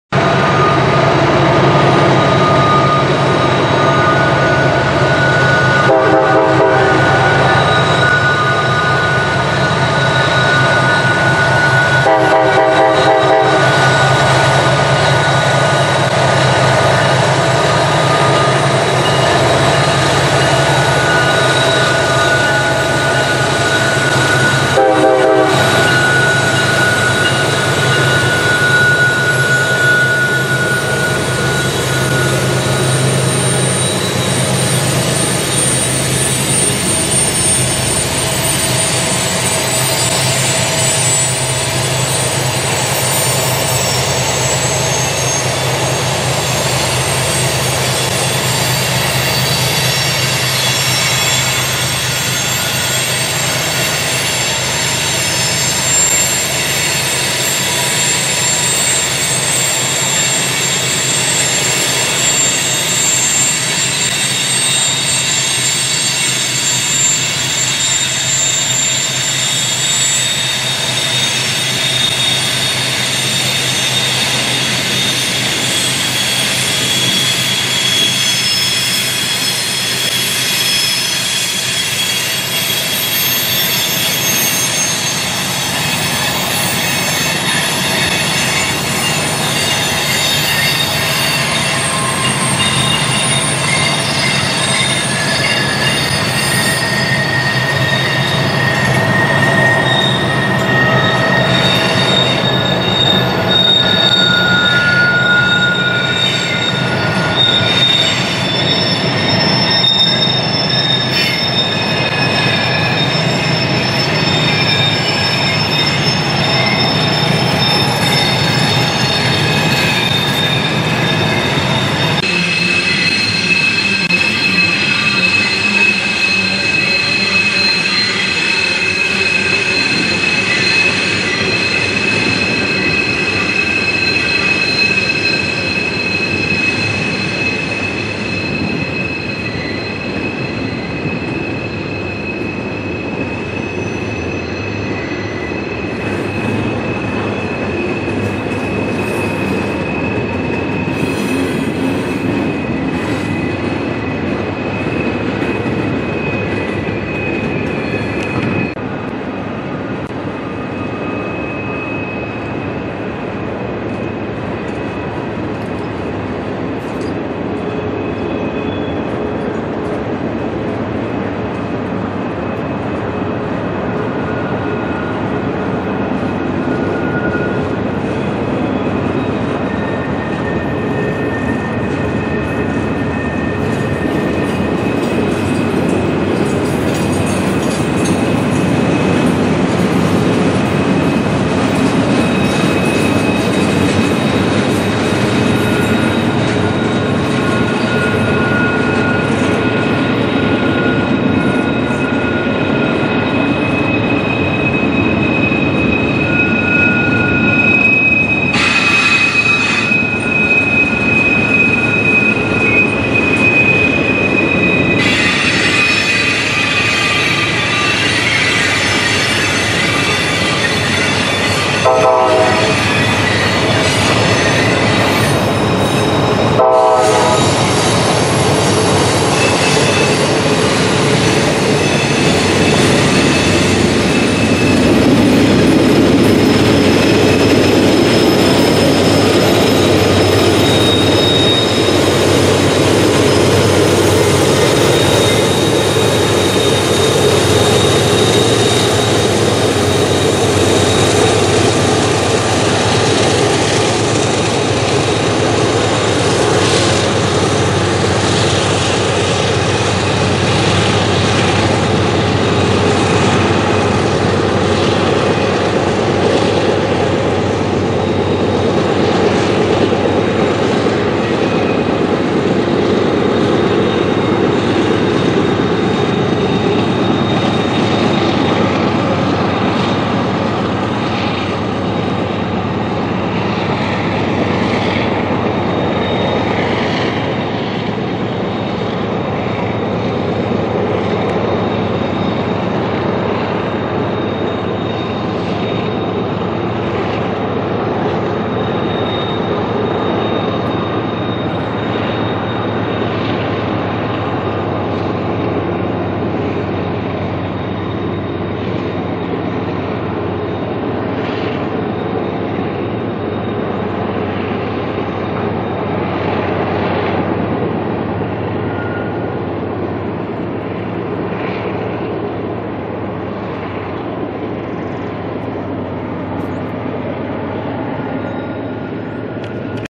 NS westbound trash train at Horseshoe Curve Altoona, PA 4-24-21
1802 EMD SD70ACC leading the train.  4490 GE AC44C6M leading the rear end helpers.